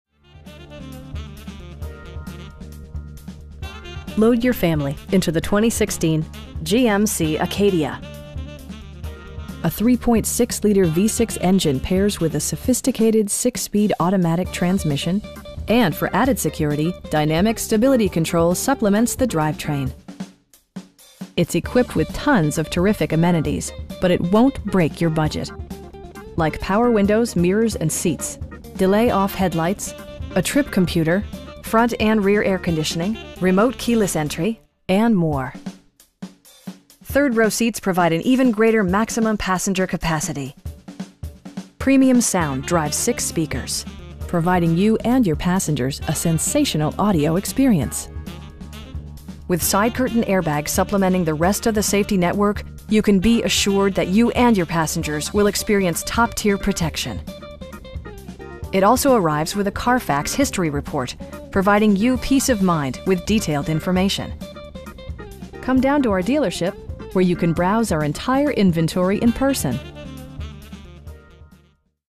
2016 GMC Acadia SLE-2 in Naperville, IL 60540
Load your family into the 2016 GMC Acadia. A 3.6 liter V-6 engine pairs with a sophisticated 6 speed automatic transmission, and for added security, dynamic Stability Control supplements the drivetrain. It's equipped with tons of terrific amenities, but it won't break your budget. Like power windows, mirrors, and seats, delay-off headlights, a trip computer, front bucket seats, front and rear air conditioning, remote keyless entry, cruise control, and more. Third row seats provide an even greater maximum passenger capacity. Premium sound drives 6 speakers, providing you and your passengers a sensational audio experience. With side curtain airbags supplementing the rest of the safety network, you can be assured that you and your passengers will experience top-tier protection. It also arrives with a Carfax history report, providing you peace of mind with detailed information. Come down to our dealership, where you can browse our entire inventory in person.